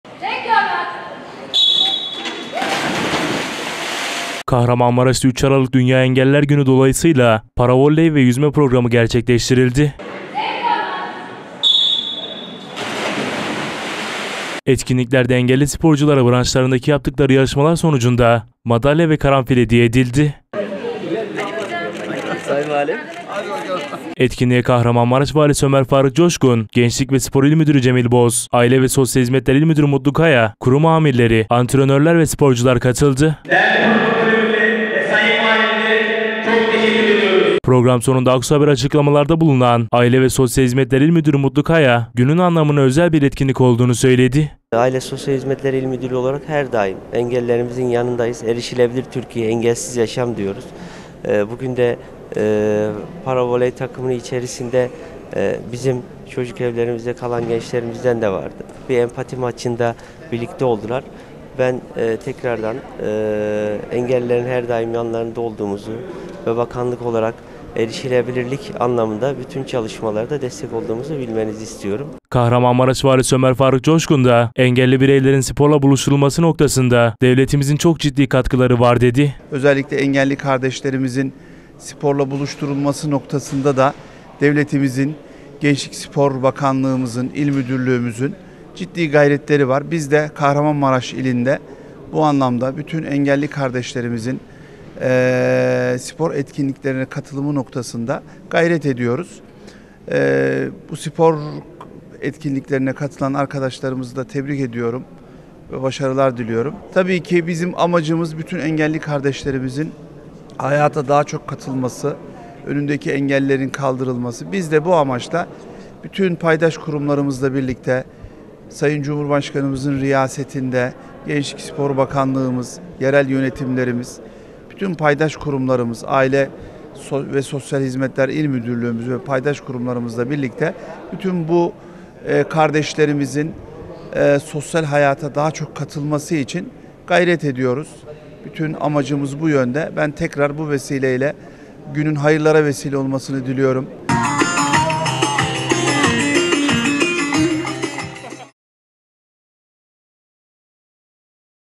Dünya Engelliler Gününe Özel Program!
Kahramanmaraş’ta 3 Aralık Dünya Engelliler Günü Dolayısıyla Paravolley Ve Yüzme Programı Gerçekleştirildi.
Etkinliklerde Engelli Sporculara Branşlarındaki Yaptıkları Yarışmalar sonucunda Madalya Ve Karanfil Hediye Edildi.
Etkinliğe Kahramanmaraş Valisi Ömer Faruk Coşkun, Gençlik Ve Spor İl Müdürü Cemil Boz, Aile Ve Sosyal Hizmetler İl Müdürü Mutlu Kaya, Kurum Amirleri, Antrenörler Ve Sporcular Katıldı.
Program Sonunda Aksu Haber’e Açıklamalarda Bulunan Aile Ve Sosyal Hizmetler İl Müdürü Mutlu Kaya Günün Anlamına Özel Bir Etkinlik Olduğunu Söyledi.
Kahramanmaraş Valisi Ömer Faruk Coşkun’da Engelli Bireylerin Sporla Buluşturulması Noktasında Devletimizin Çok Ciddi Katkıları Var Dedi.